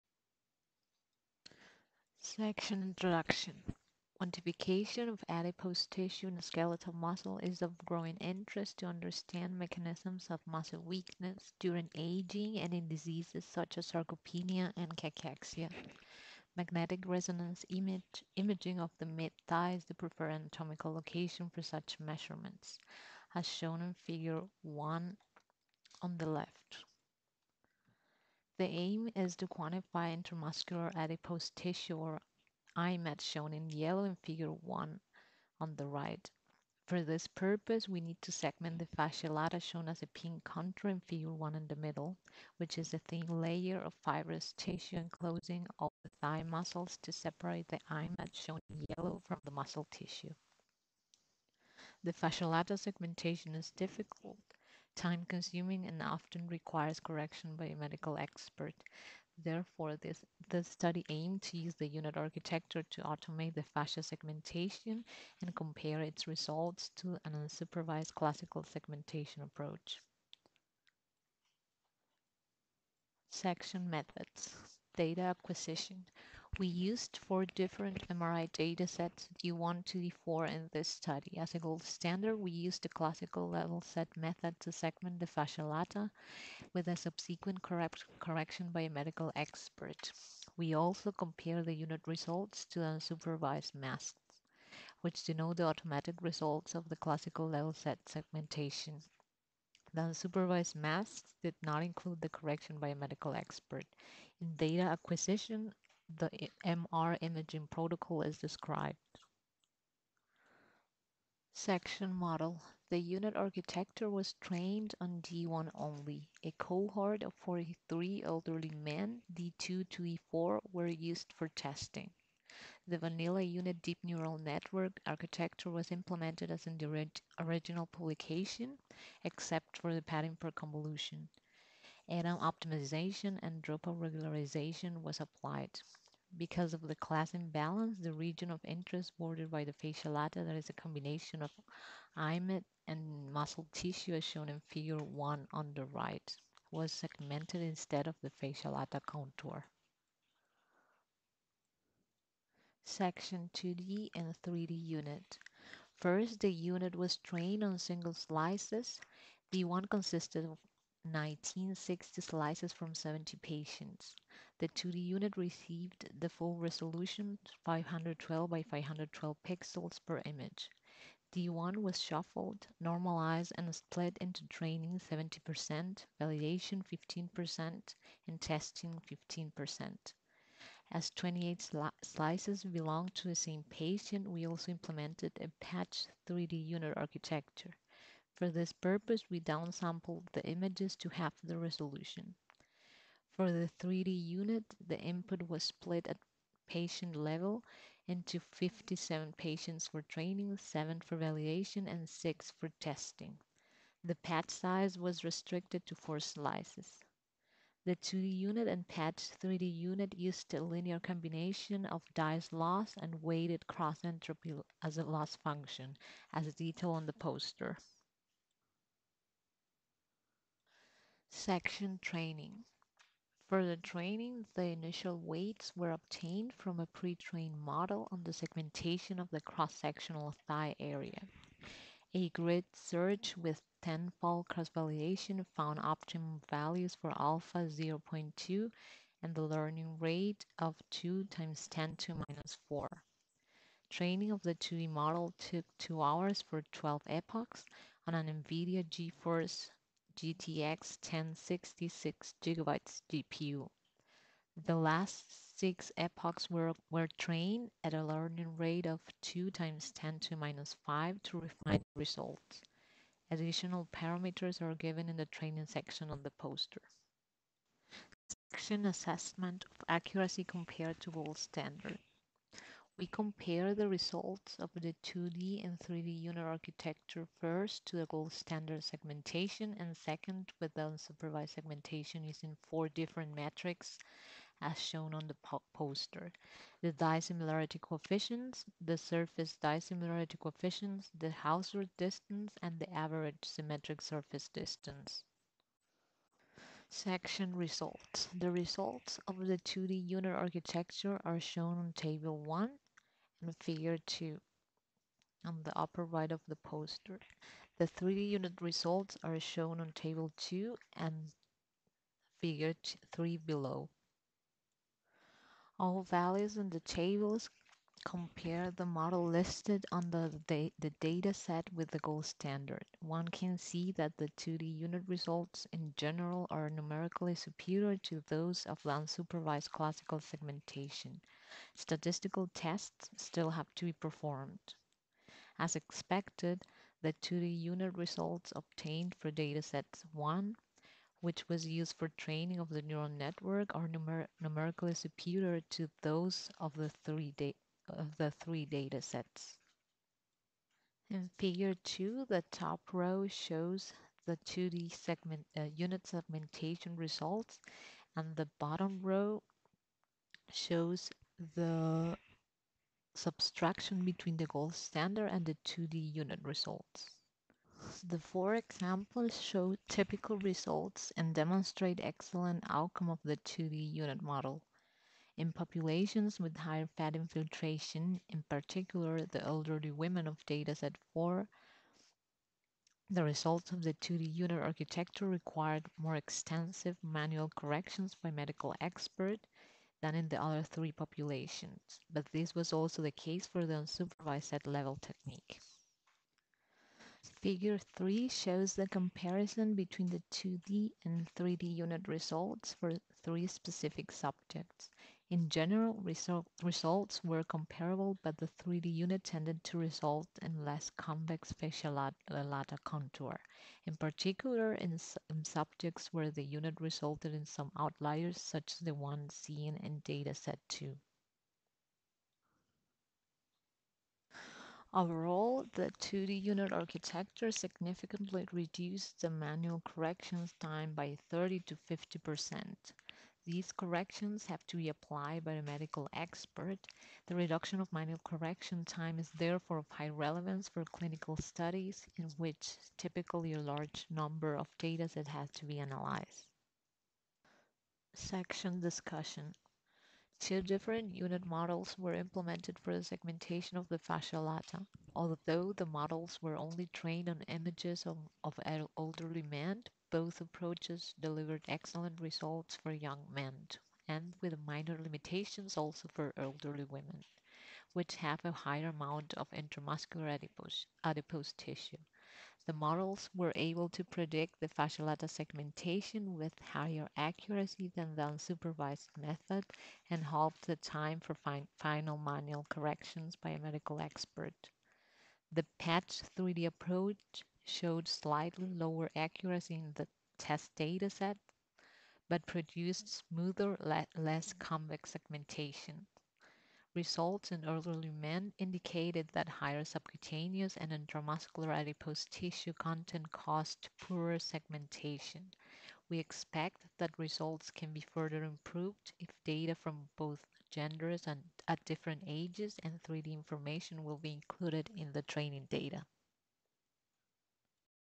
Segmentation of the Fascia Lata in Magnetic Resonance Images of the Thigh
Segmentation of the Fascia Lata in Magnetic Resonance Images of the Thigh: Comparison of an Unsupervised Technique with a U-Net in 2D and Patch-wise 3D

Abstract

To quantify muscle properties in the thigh, the segmentation of the fascia lata is crucial. For this purpose, the U-Net architecture was implemented and compared for 2D images and patched 3D image stacks in magnetic resonance images (MRI). The training data consisted of T1 MRI data sets from elderly men. To test the performance of the models, they were applied on other data sets of different age groups and gender. The U-Net approaches were superior to an unsupervised semiautomatic method and reduced post-processing time.

References:
Louise P L.J., Engelke K., Chaudry O. (2021) Segmentation of the Fascia Lata in Magnetic Resonance Images of the Thigh. In: Palm C., Deserno T.M., Handels H., Maier A., Maier-Hein K., Tolxdorff T. (eds) Bildverarbeitung für die Medizin 2021. Informatik aktuell. Springer Vieweg, Wiesbaden.